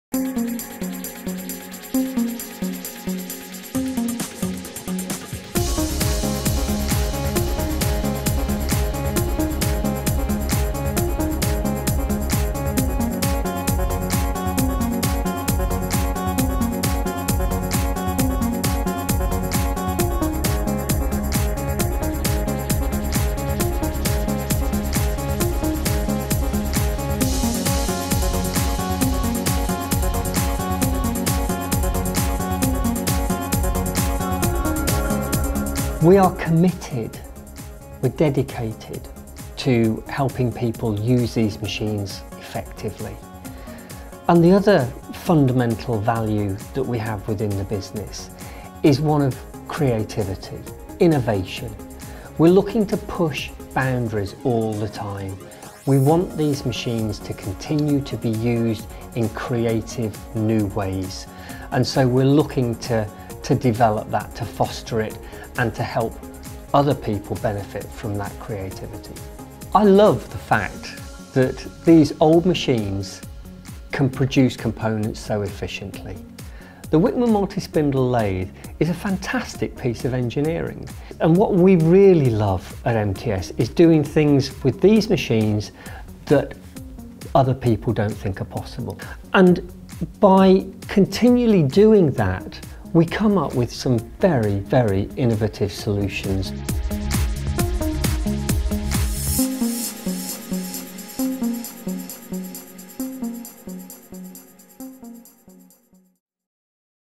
Wickman multispindle lathes: Shaping the future with innovative process development
Through advanced engineering, Machine Tool Spares revolutionsises the use of Wickman multispindle lathes, identifying a powerful role for this technology in the twenty-first century. In this video, David Taylor, Managing Director of Machine Tool Spares reiterates the advantages of the Wickman multispindle lathe, the benefits of developing specific turnkey solutions and the value of the customised attachment services on offer at Machine Tool Spares.